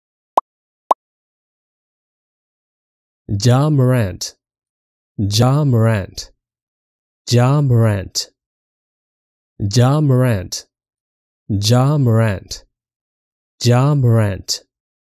How to Pronounce Ja Morant
In this video we'll show you how to correctly pronounce:
Ja Morant.